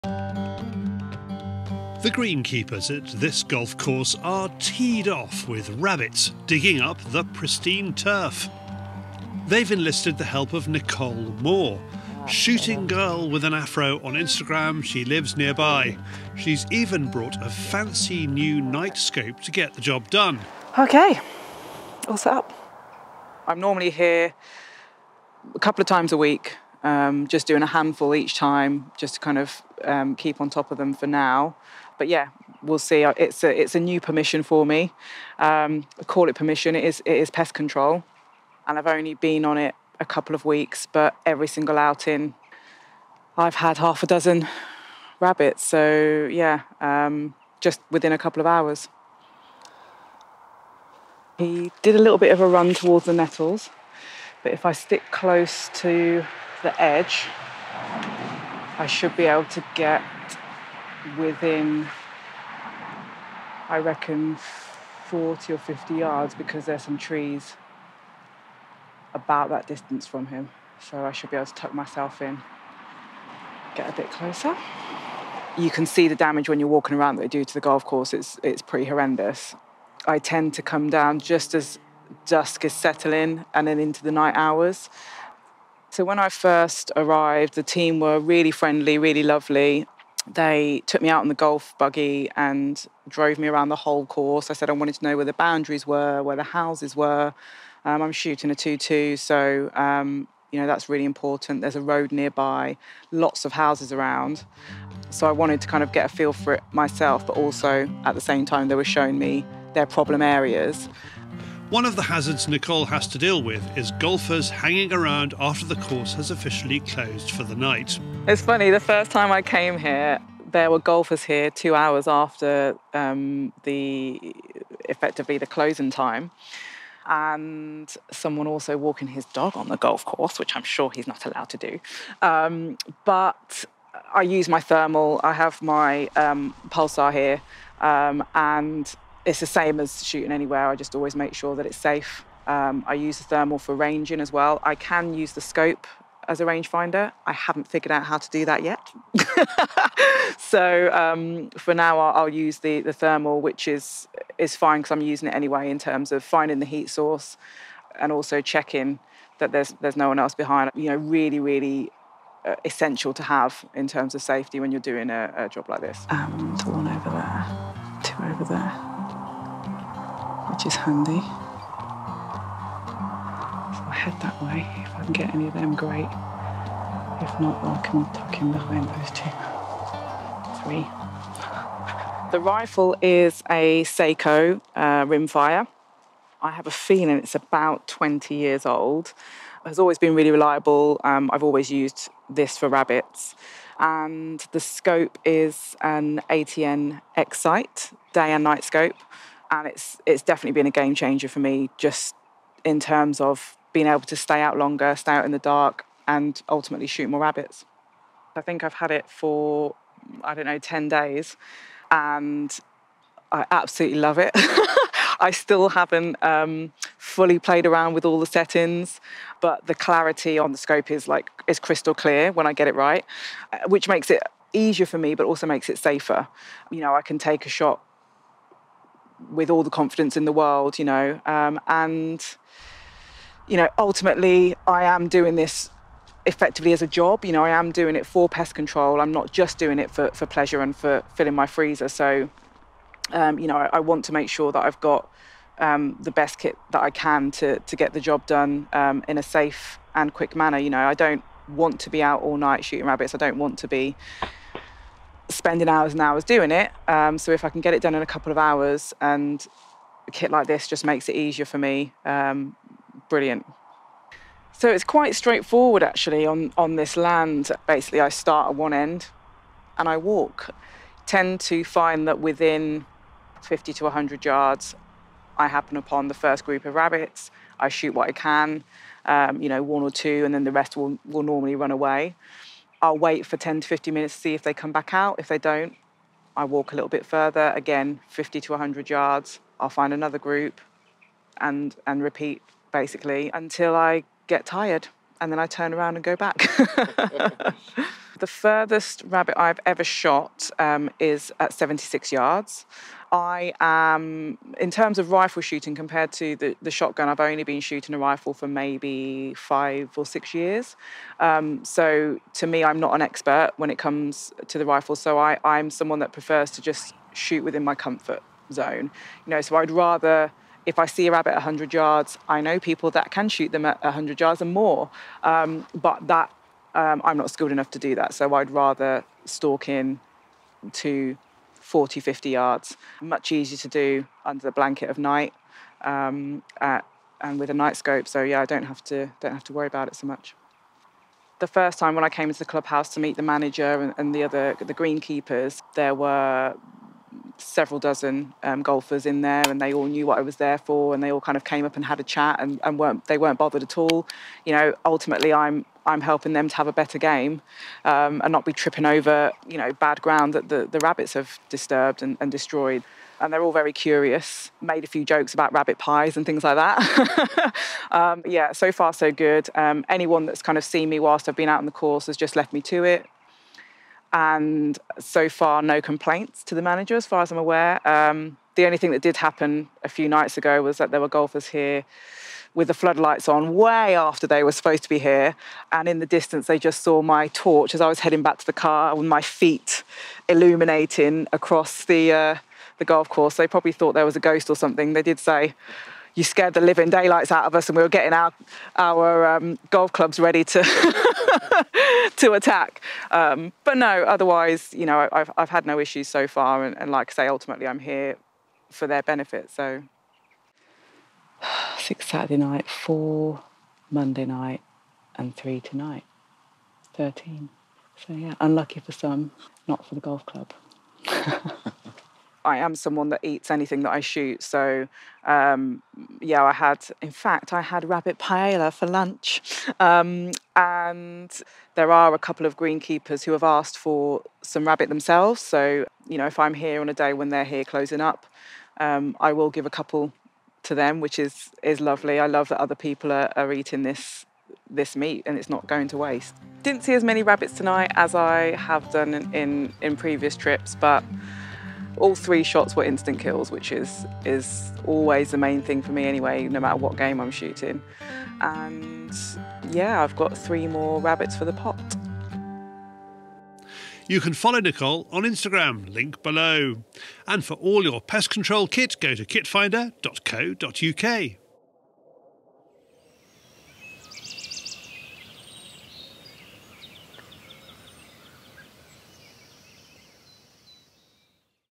Golf course rabbit control
Nicole Moore, Instagram's Shooting Girl with an Afro, has taken on rabbit control duties at a golf course close to her home in Norfolk, where the rabbits are causing terrible damage to the greens. She's using a .22 Sako rifle, subsonic ammo, and she has got a new ATN X-Sight day/night scope so she can shoot in darkness once the golfers have gone home.